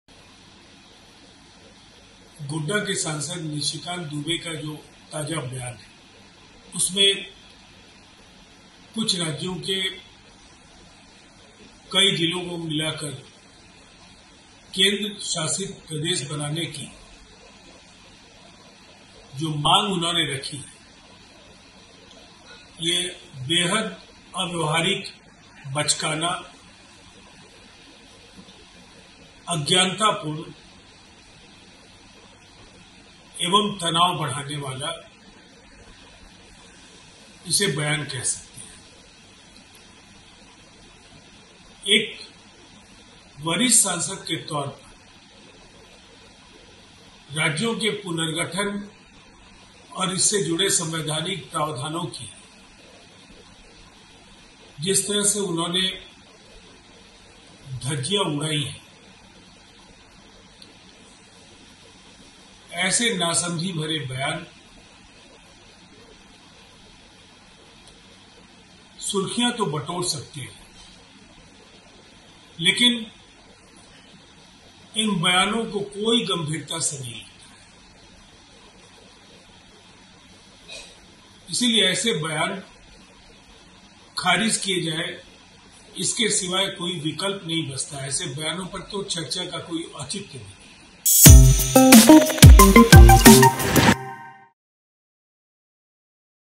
Nishikant Dubey के इस बयान पर भड़के नीतीश के नेता, बताया- ‘बचकाना और....’
झारखंड से बीजेपी सांसद निशिकांत दुबे के बयान से जेडीयू सहमत नहीं है. उनके बयान पर जेडीयू के राष्ट्रीय सचिव राजीव रंजन ने (27 जुलाई) को तीखी प्रतिक्रिया दी है. उन्होंने कहा कि निशिकांत दुबे कुछ राज्यों के कई जिलों को मिलाकर केंद्र शासित प्रदेश बनाने की मांग जो वह कर रहे हैं, इसे बेहद अव्यवहारिक, बचकाना, अज्ञानता पूर्ण, तनाव बढ़ाने वाला बयान कह सकते हैं…